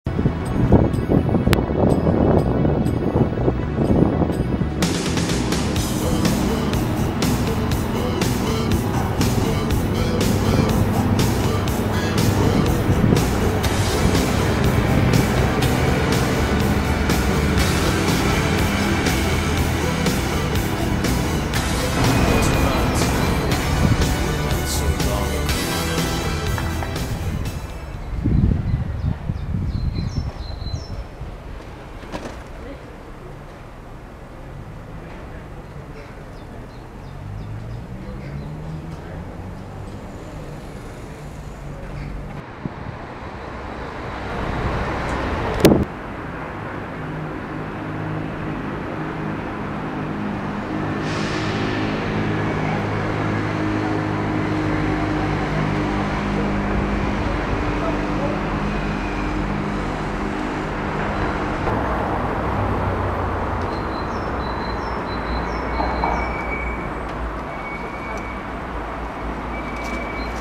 climbing
bit of climbing. Its a parking garage. The metal isn't that strong thus corners were the only handholds that could hold my weight. In the middle the plates started to bend.
The top is a bit wobly. There are precision jumps at different heights and there is a small outpost getting you even higher up.

This climb was rather easy, more like a ladder but still giving plenty of opportunities.